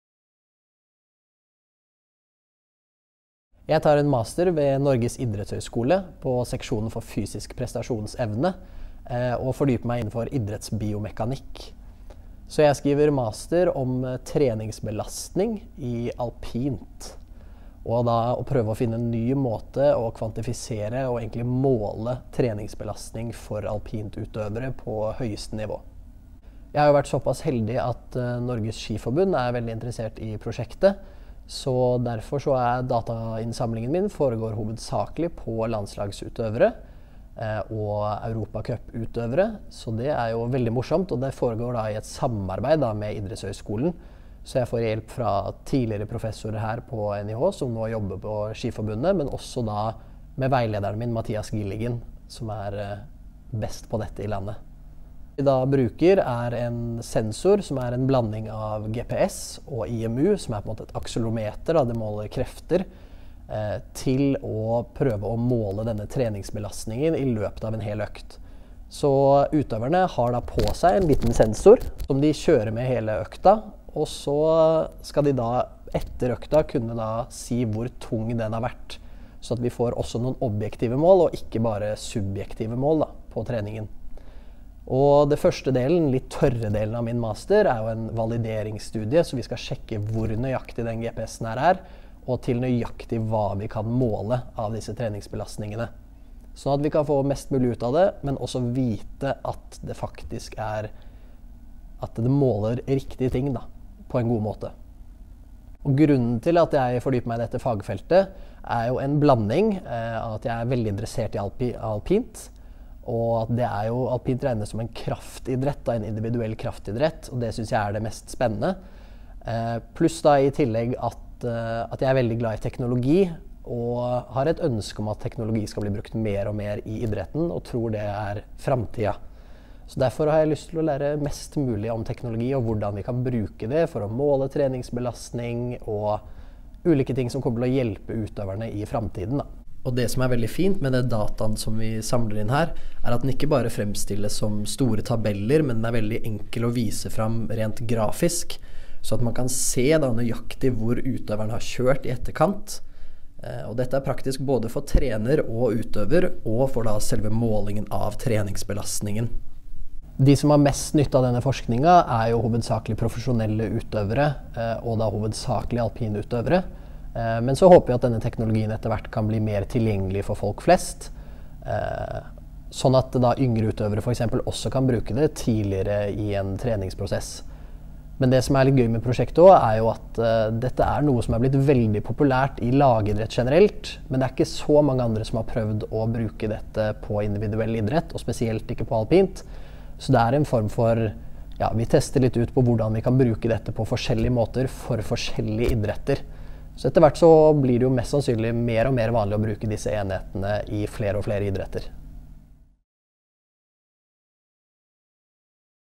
Masteroppgave om treningsbelastning i alpint
Petter Andrè Jølstad tar en master i idrettsvitenskap, studieretning fysisk prestasjonsevne. Masteroppgaven kombinerer teknologi og idrett, og handler om treningsbelastning i alpint, og hvordan blant annet GPS og IMU kan brukes for å måle belastningen til utøverne.